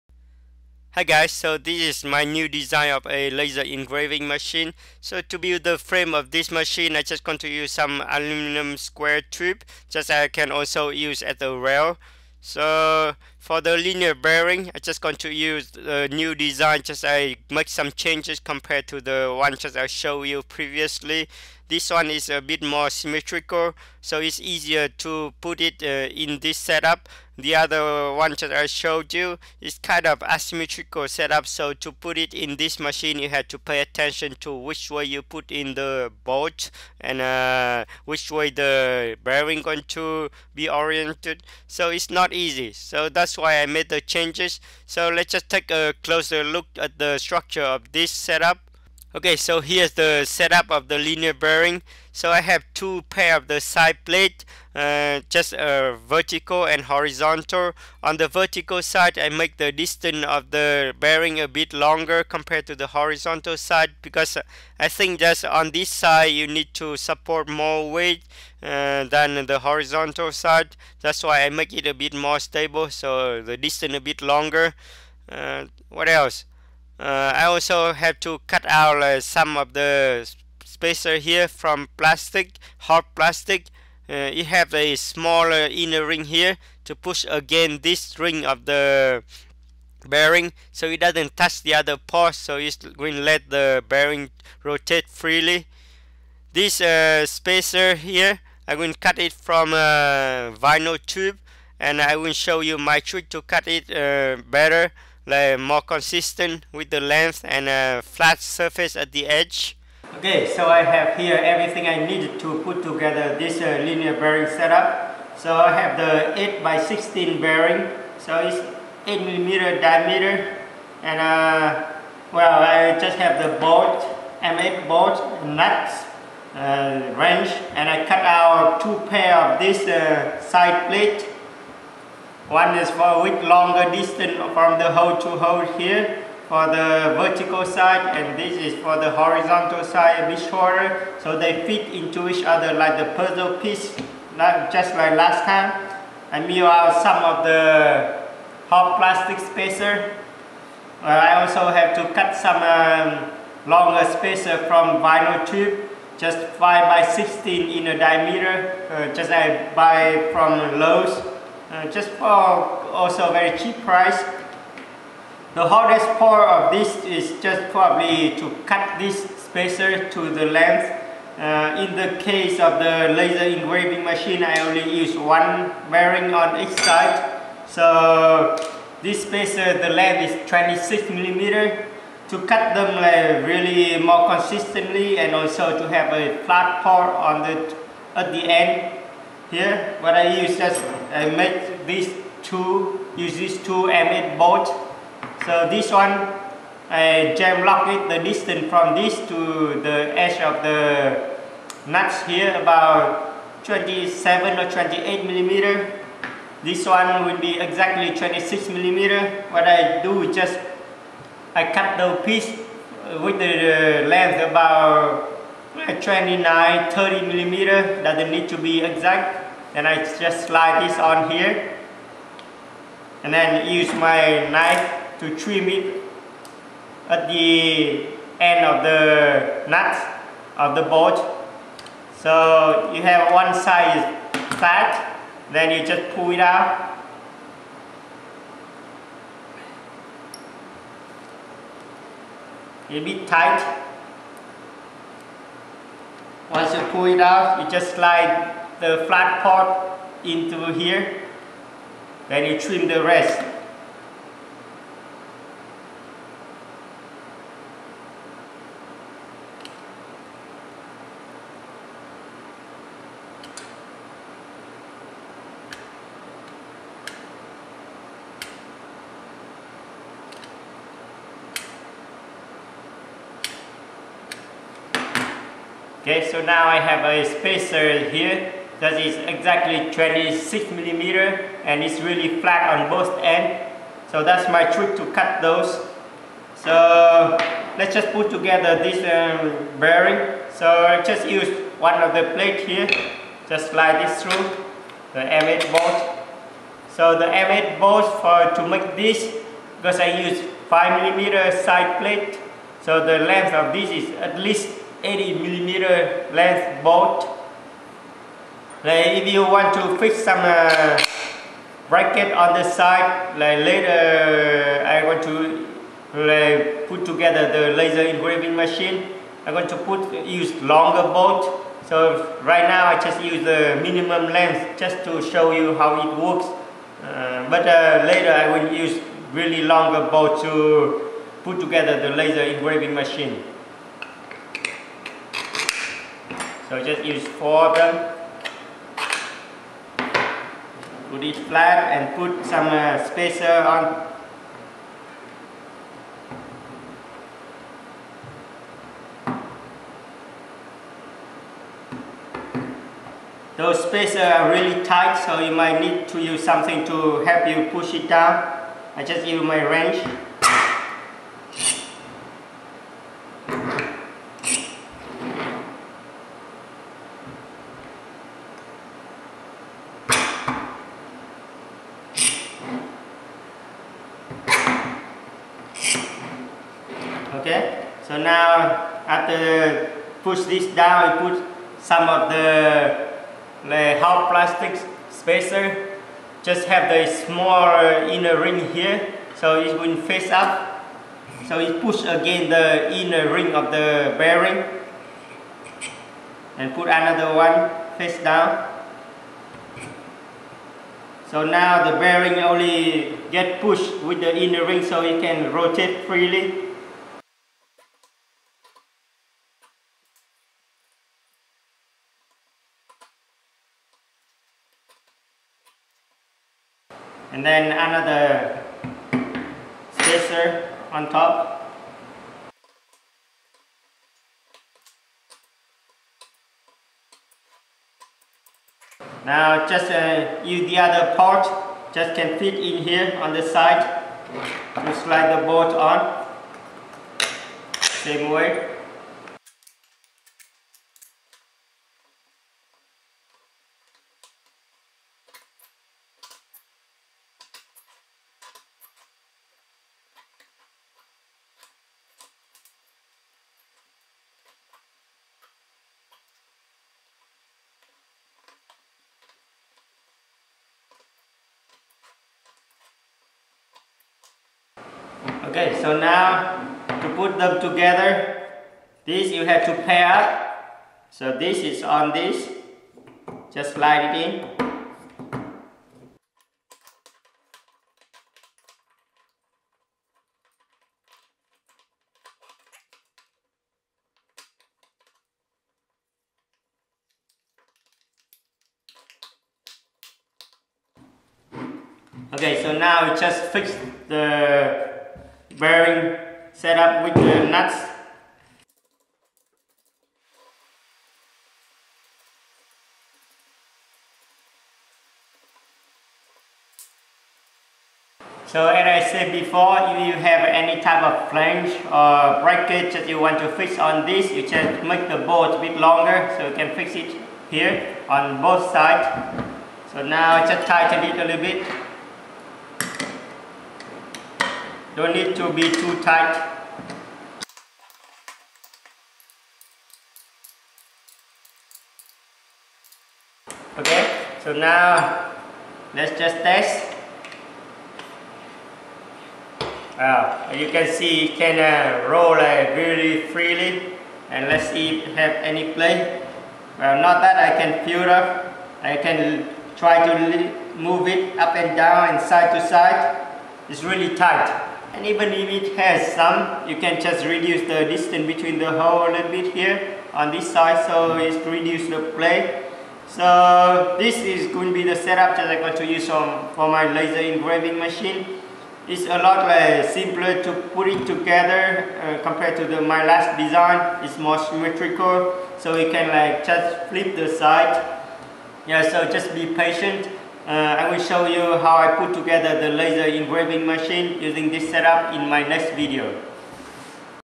Linear bearing for square tube V2 - symmetrical design
While designing my new laser engraving CNC, I realized that my previous design of linear bearing wasn't symmetrical:

That made it quite difficult to integrate the bearing into the laser engraver. So I made some changes to the linear bearing to make it a lot simpler to assemble and also to integrate into any CNC machine design.
The new design works as good as the previous one and has more advantages in simplicity and symmetries.